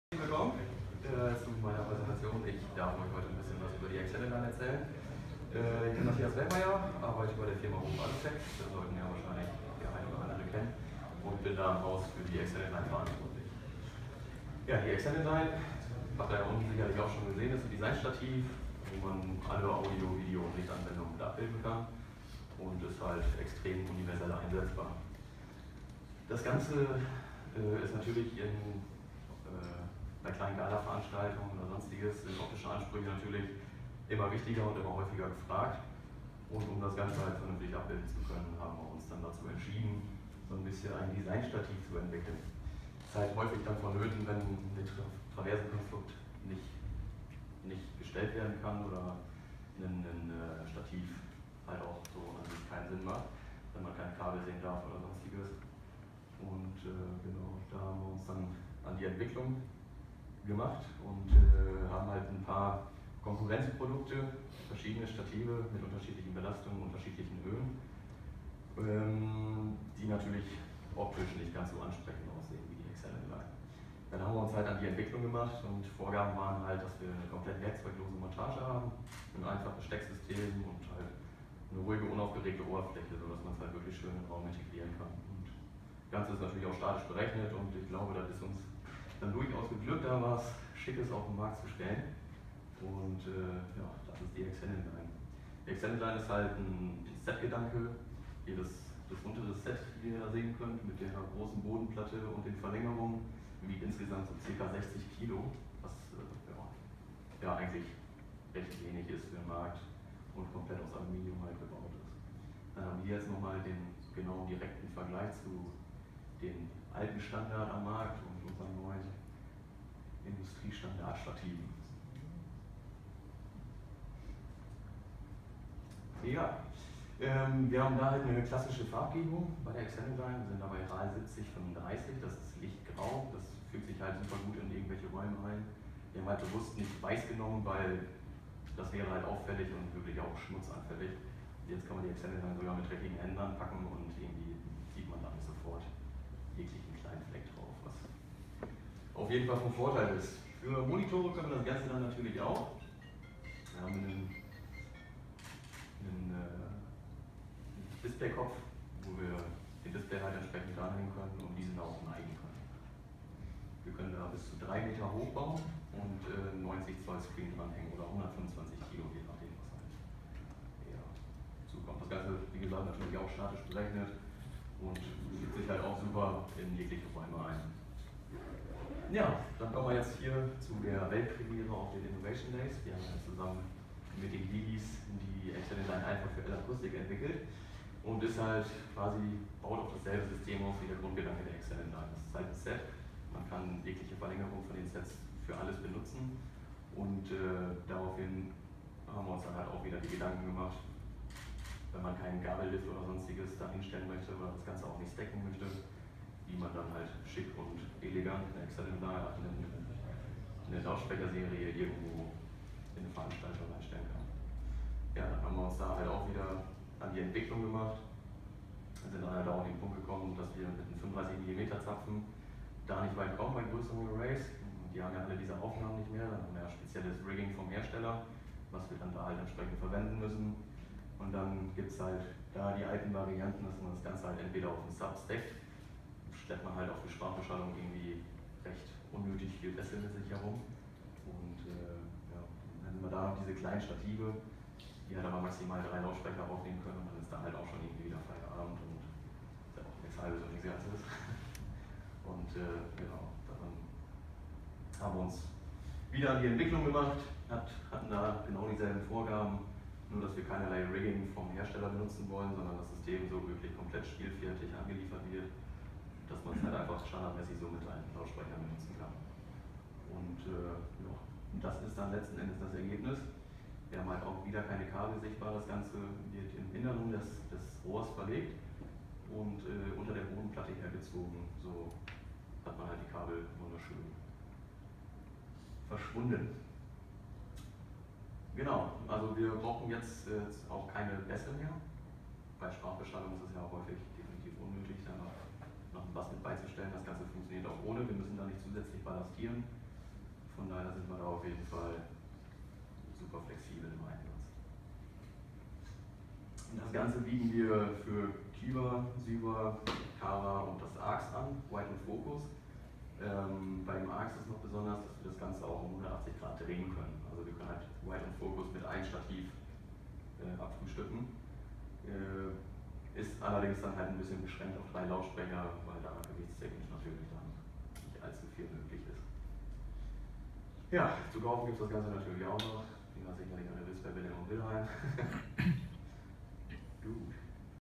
Funktionaler Purismus - das Motto der Excellent Line von HOF
Matthias Wellmeyer von HOF Alutec zur Excellent Line und besonders zu den brandneuen Halterungen für die Lautsprechersysteme von L-Acoustics